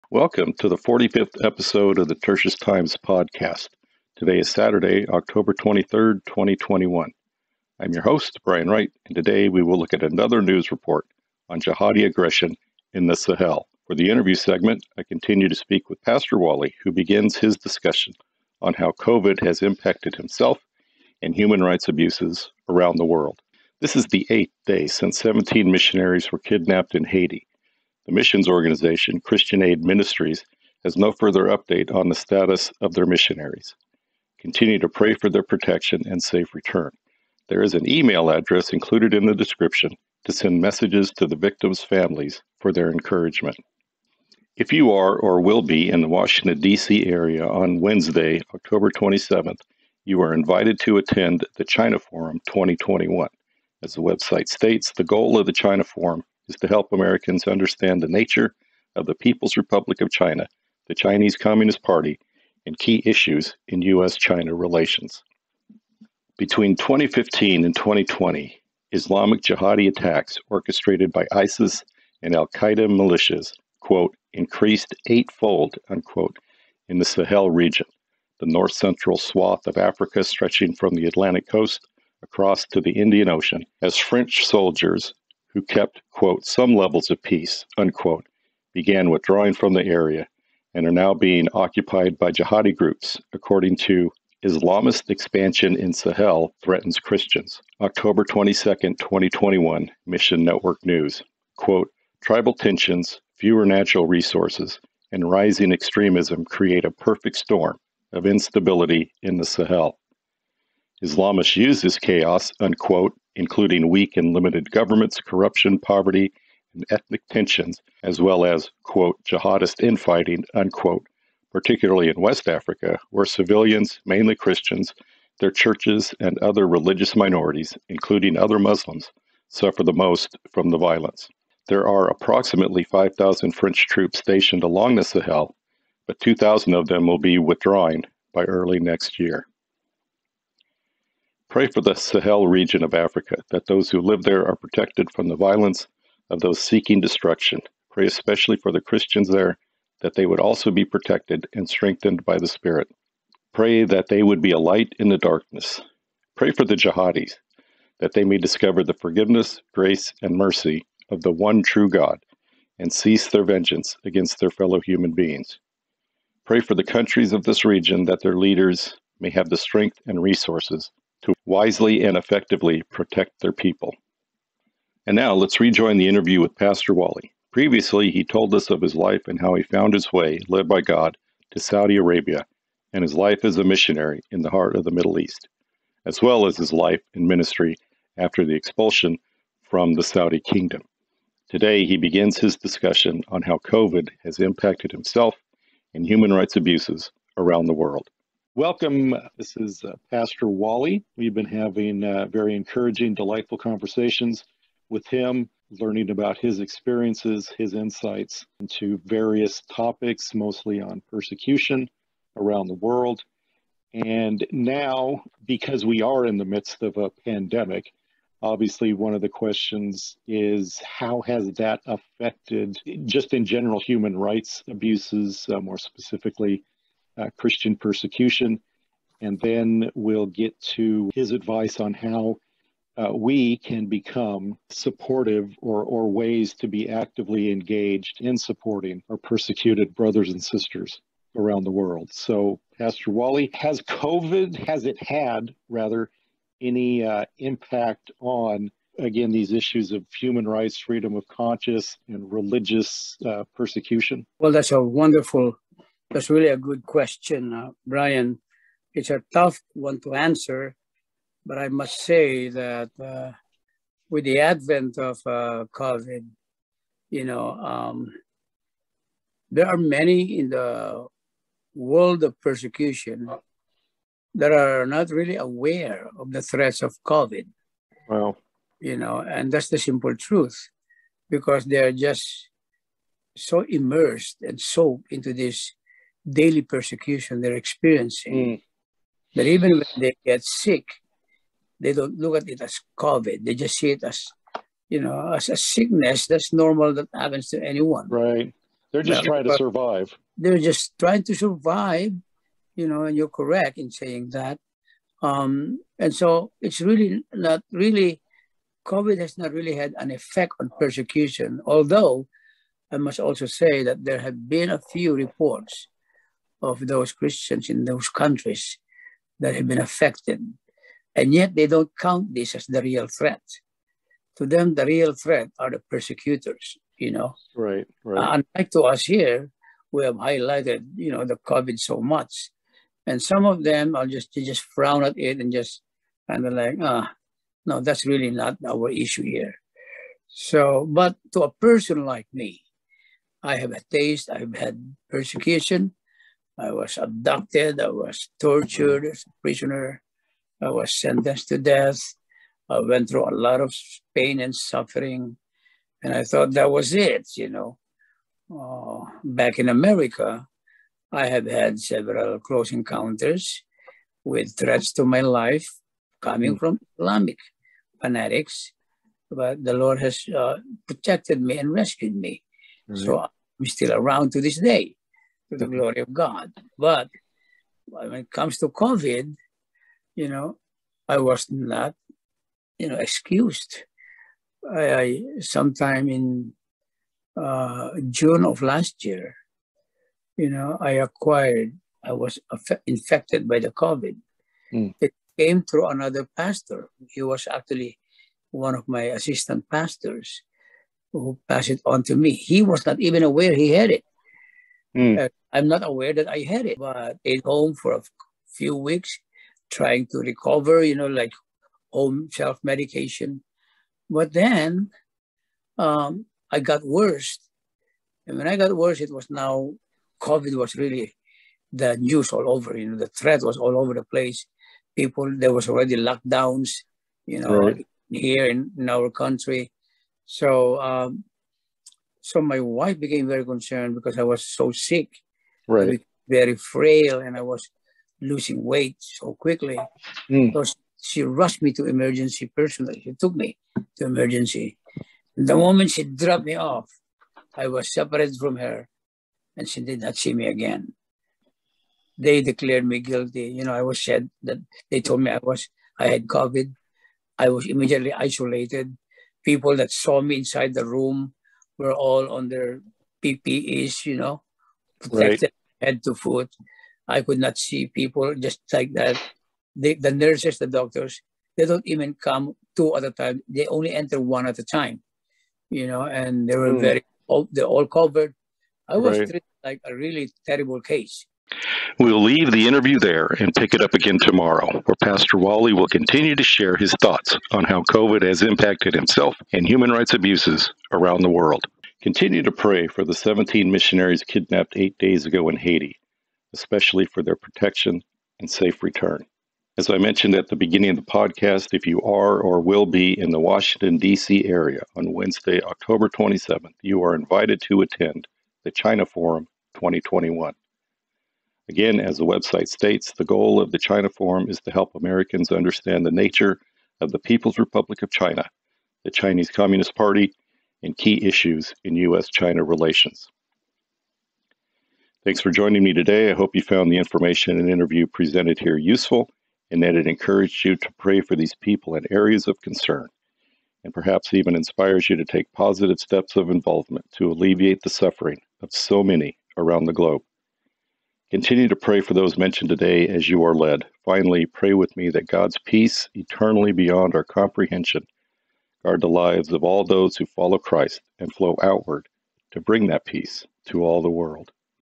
Tertius Times Episode 45 Oct 23 2021 Full
Here are the stories and links in today’s podcast:
Update on Haiti kidnapping (Oct. 22, 2021, Christian Aid Ministries)

"Islamist expansion in Sahel threatens Christians" (Oct. 22, 2021, Mission Network News)